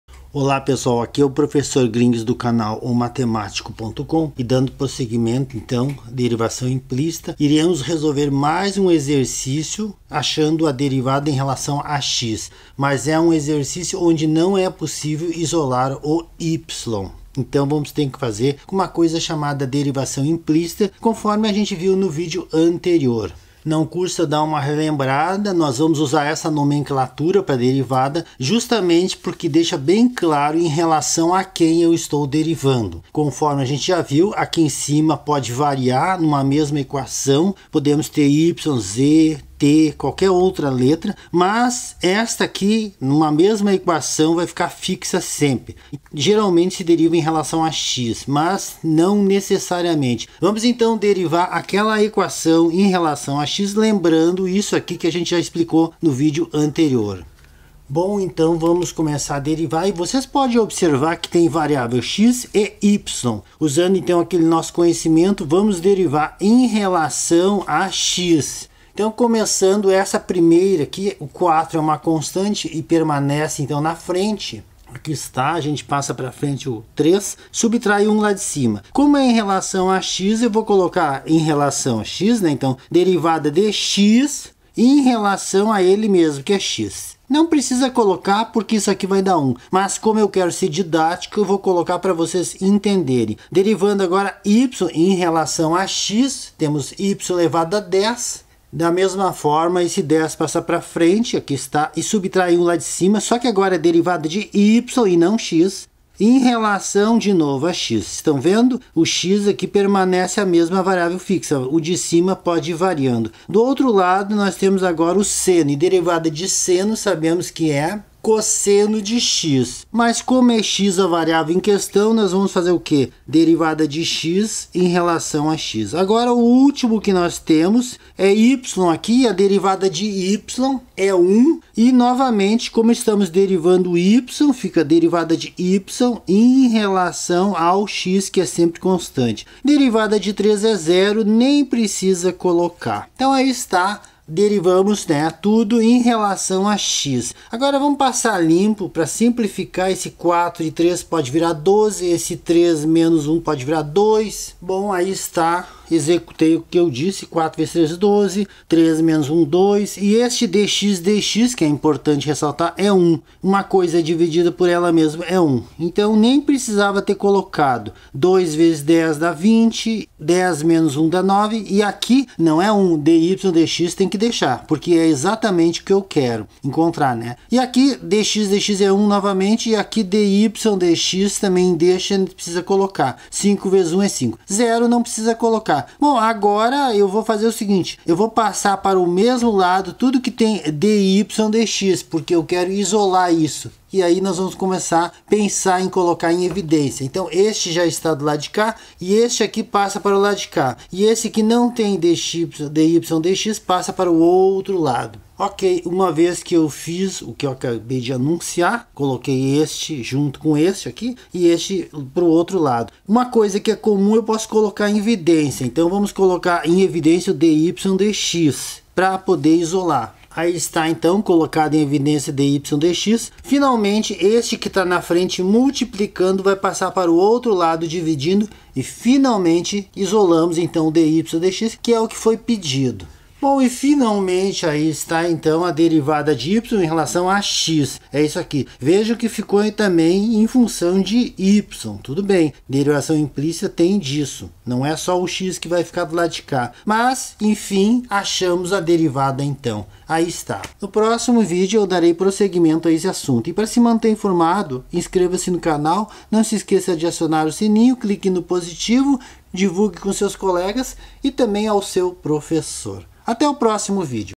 GRINGS - Derivação ou Diferenciação Implícita - Aula 2
Segundo vídeo de Derivação ou diferenciação implícita. Exercício detalhadamente explicado.

ESTUDAR  nunca  foi  tão  fácil!